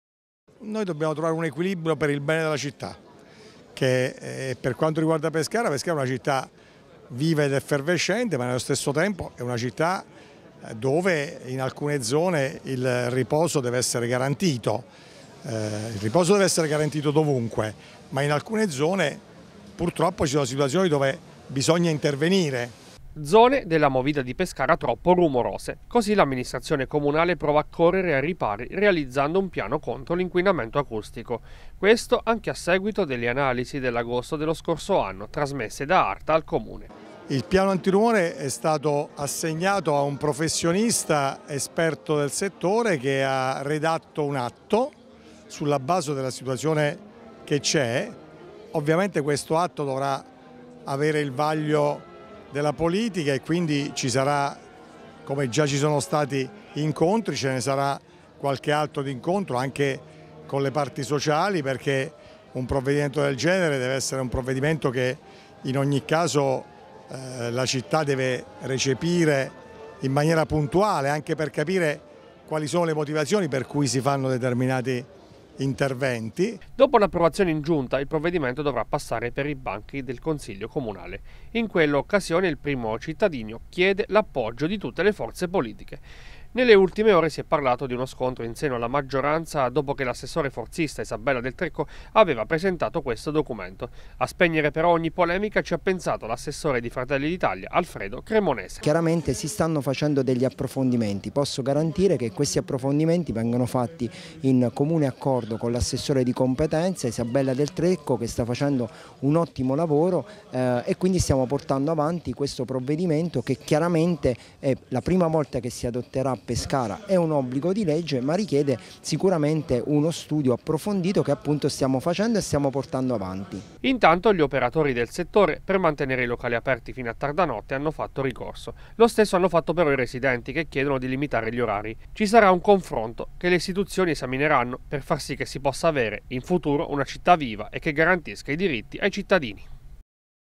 Pescara. Pronto il piano anti-rumore per limitare la movida
Come da decreto legge anche la città di Pescara dovrà dotarsi del piano anti-rumore. L’amministrazione è al lavoro per approvarlo in breve tempo anche in vista della stagione estiva. Ma non sarà facile accontentare gestori dei locali da un lato e residenti dall’altro